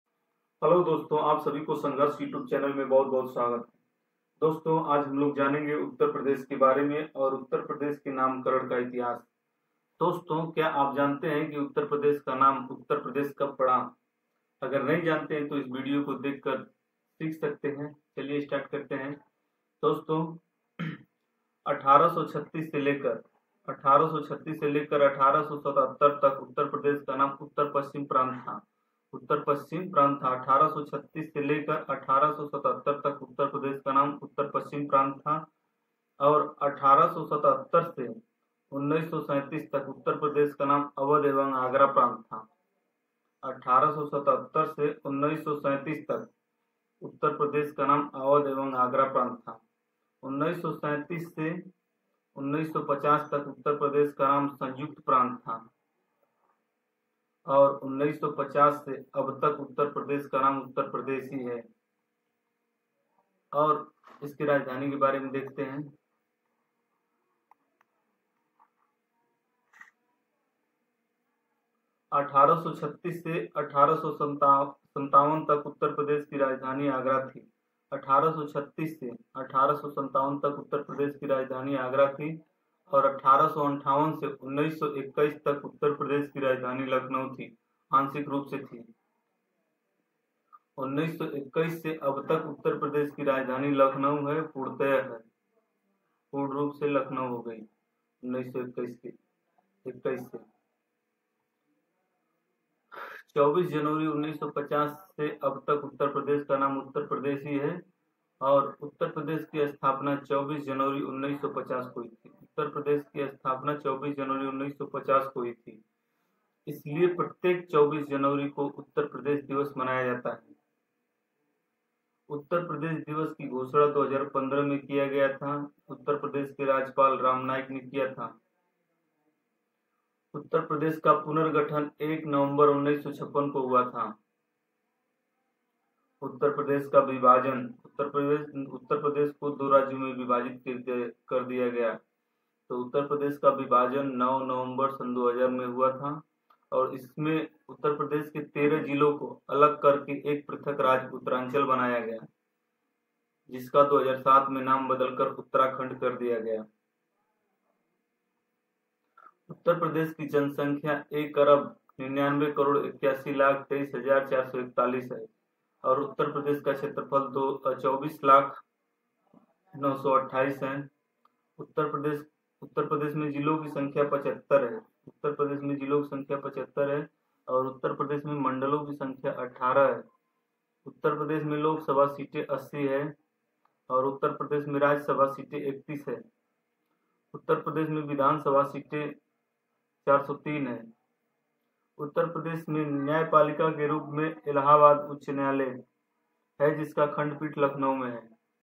Uttar Pradesh ka nam karan उत्तर प्रदेश का नाम उत्तर प्रदेश कब हुआ । About Uttar Pradesh
Sangharsh Dinesh Kumar
dineshkumar Sangharsh
About Uttar Pradesh